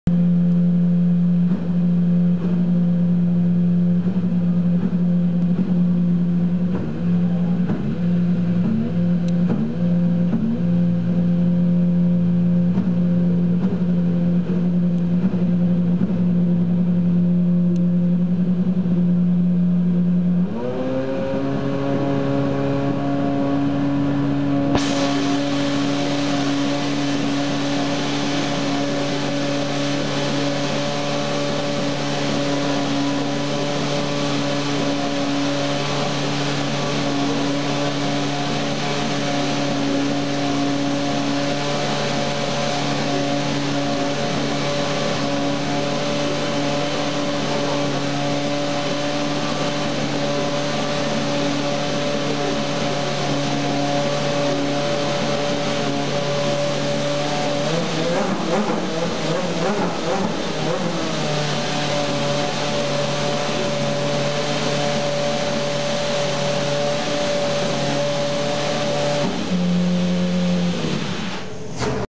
Ginetta-Zytek 09S engine warming up - (Quifel ASM Team) Miguel Amaral/Olivier Pla
before the 1000km of Hungaroring LMS race